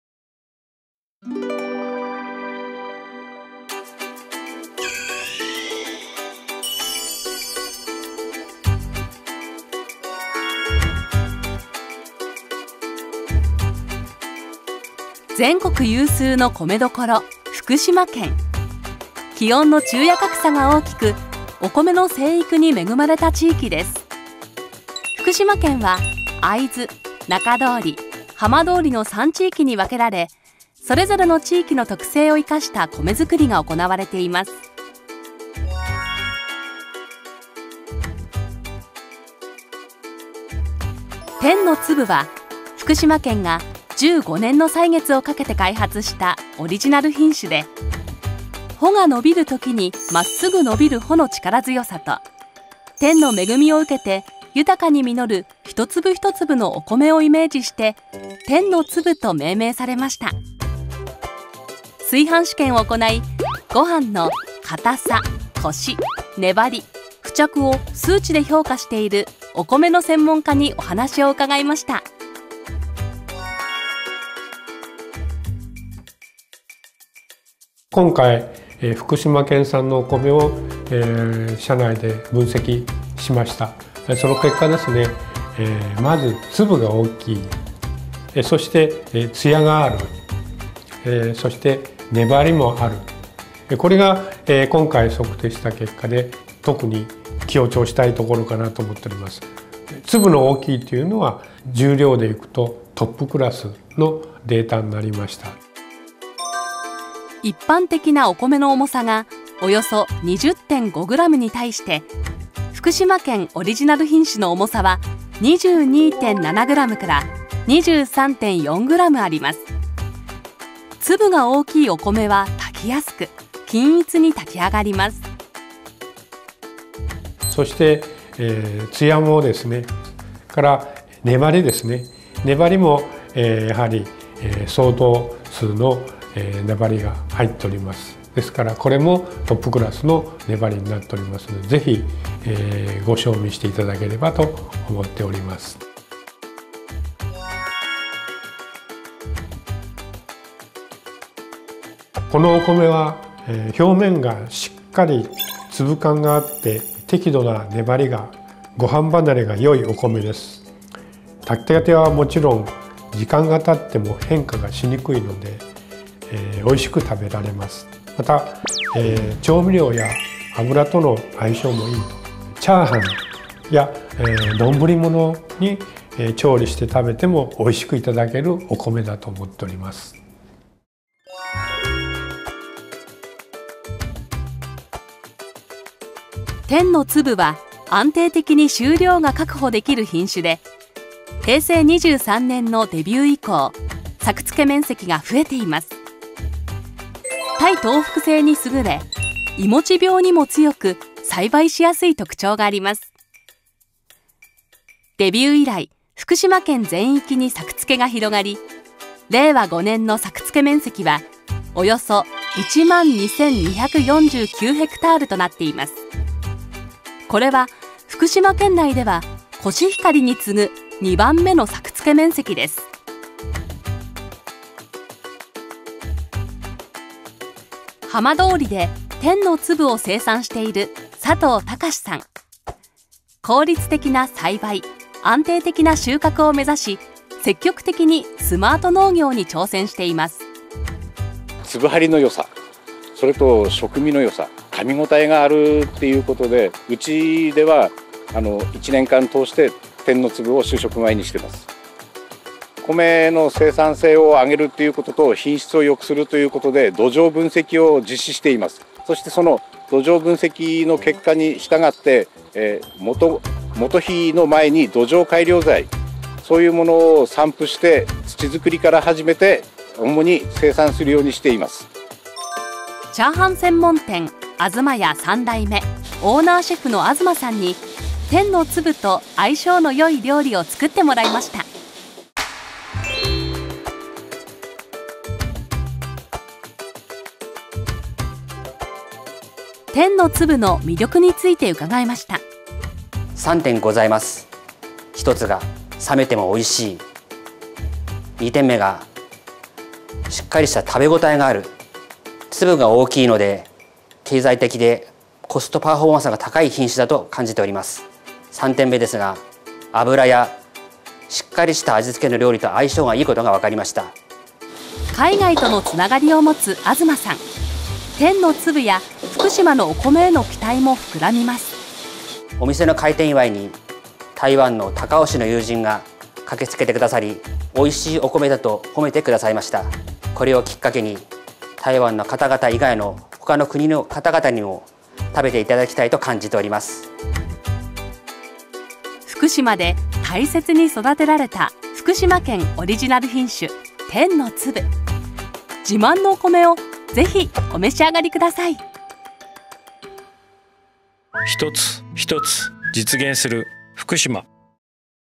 福島県オリジナル品種 天のつぶ
◆この動画は福島県オリジナル品種「天のつぶ」を紹介する動画です。

◆福島県産米全体の生産量などのデータ、生産者の声のほか、株式会社AIHOの鈴木顧問が、ご飯の硬さ、コシ、粘り、付着などの要素などからオリジナル品種の炊飯後の特徴をご紹介しております。

◆また、県内飲食店によるオススメの食べ方や使用感もご紹介しておりますので、ぜひご家庭などでお試しください。
　「天のつぶ」は、いわき市の「炒飯専門店東や３代目」オーナーシェフの東氏に納豆ラー油炒飯にしていただきました。